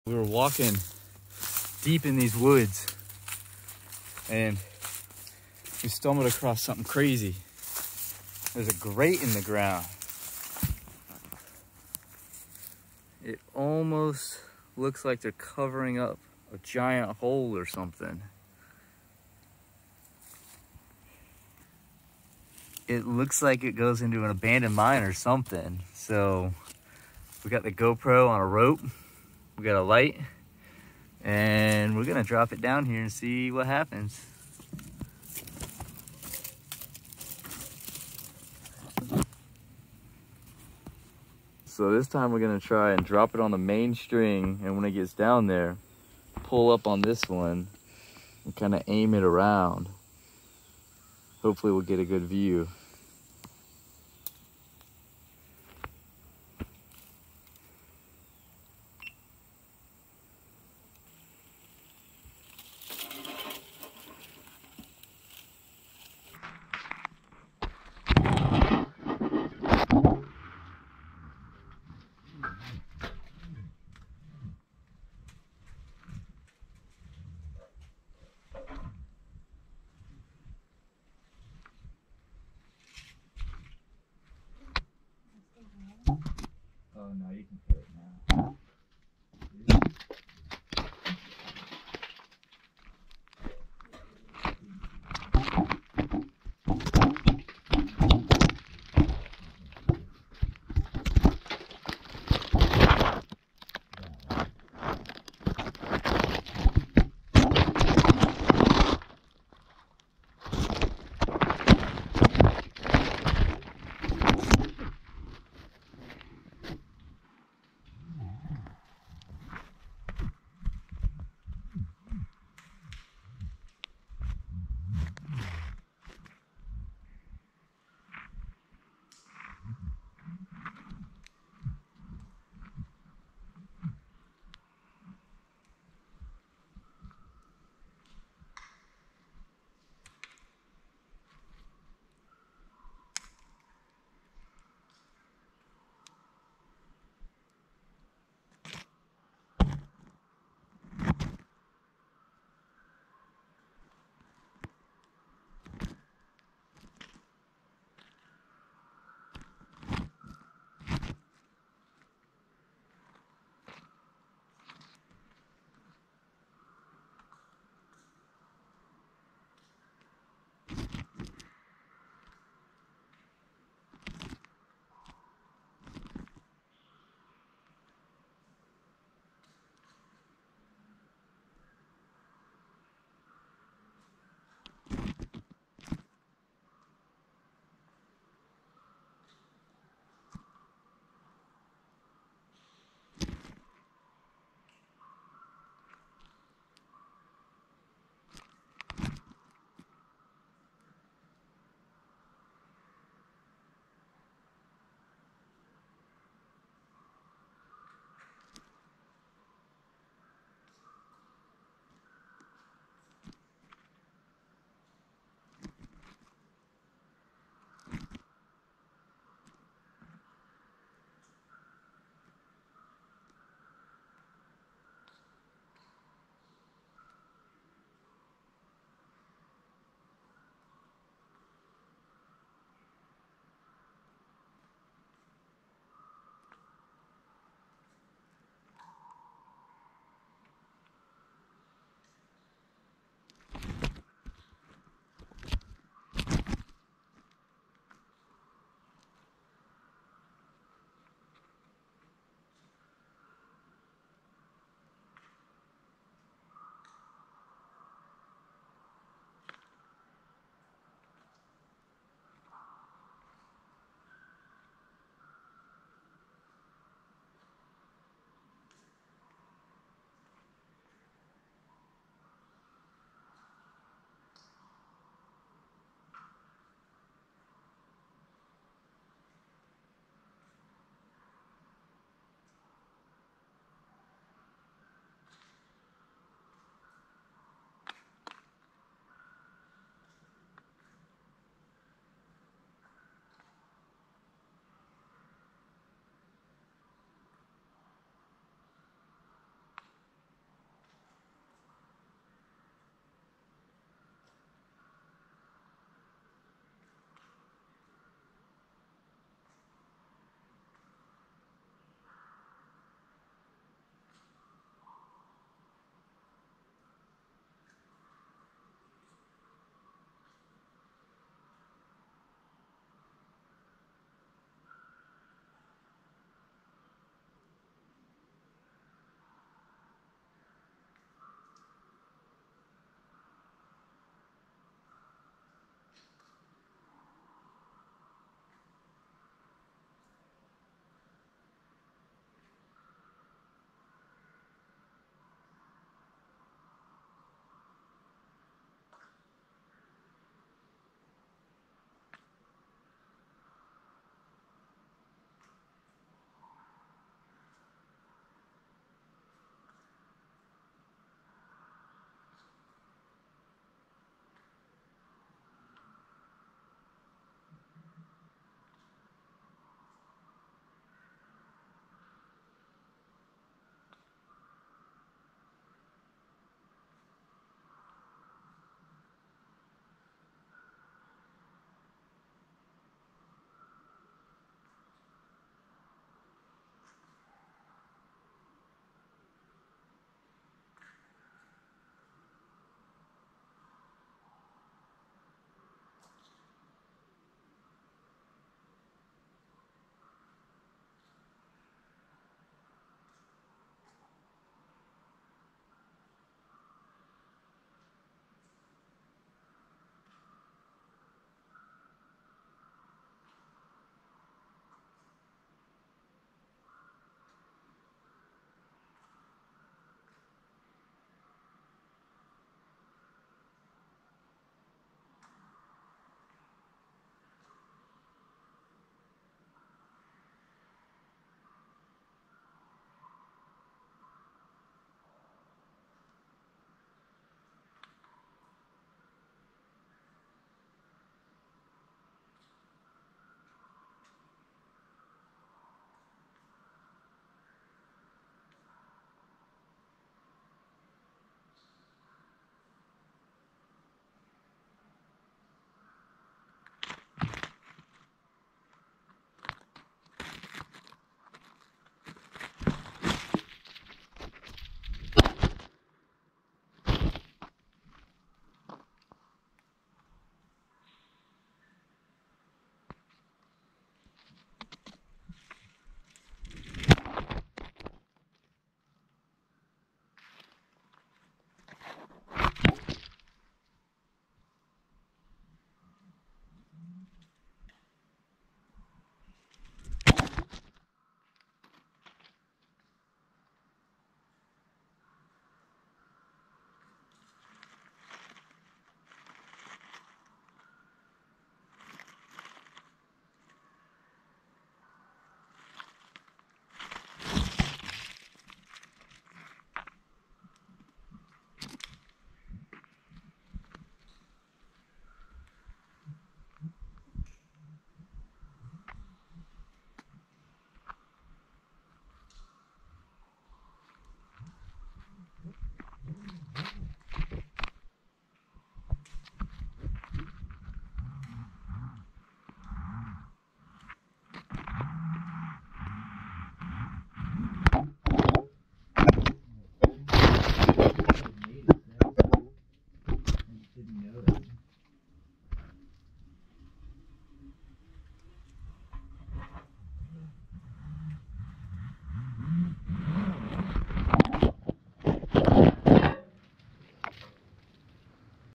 Go Pro Dropped Into A Hidden Mineshaft
We discovered a metal grate covering a very deep hole on the side of a mountain and dropped a go pro camera down into it and can't believe where it ended up.